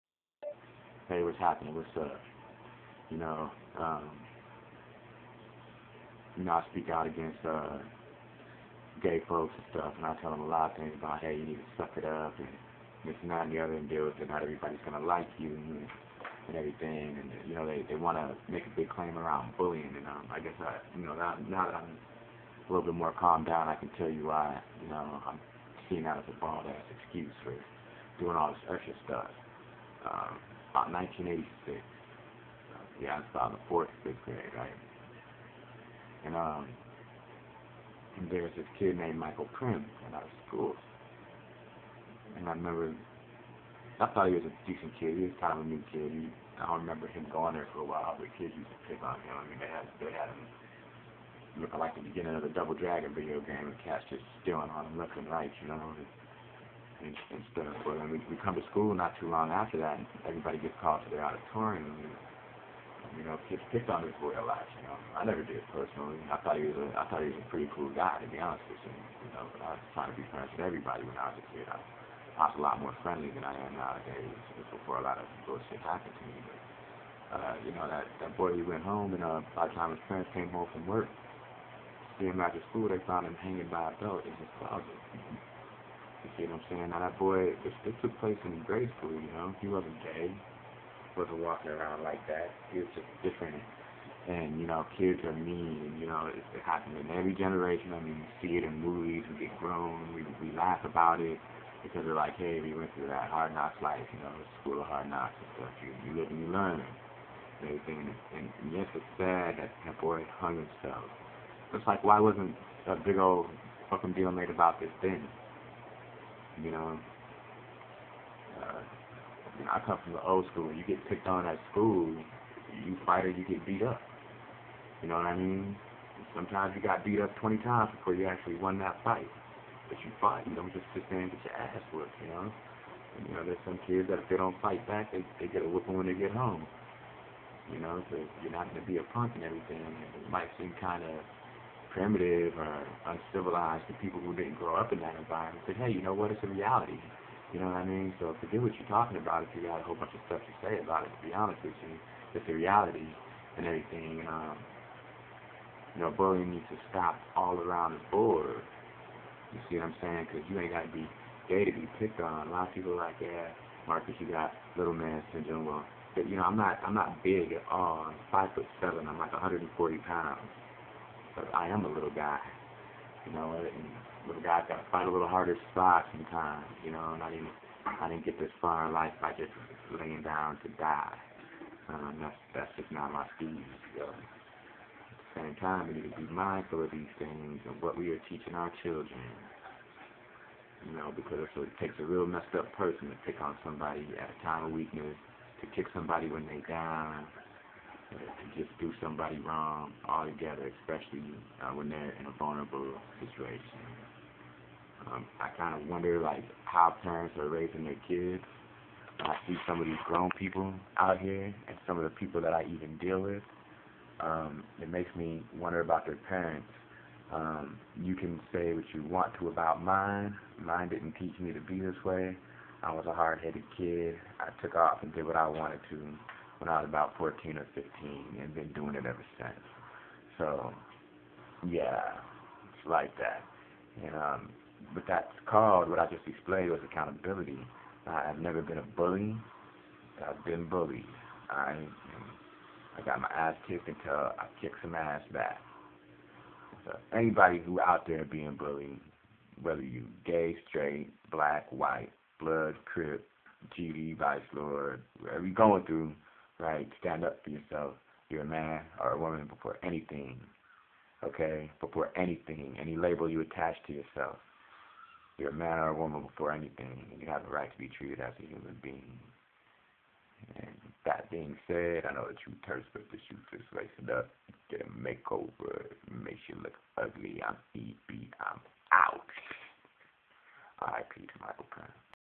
EB Speaks A true story about bullying-2011-08-12
Now this is about when i was in grade school. This is what i know about bullies. This is before people made a big deal about it. No the kid wasnt gay, we were in like the 4th grade.
The fact is this, bullies been around forever. If you want to put an end to it, do make such a big deal about one group of people. Equality is for everyone.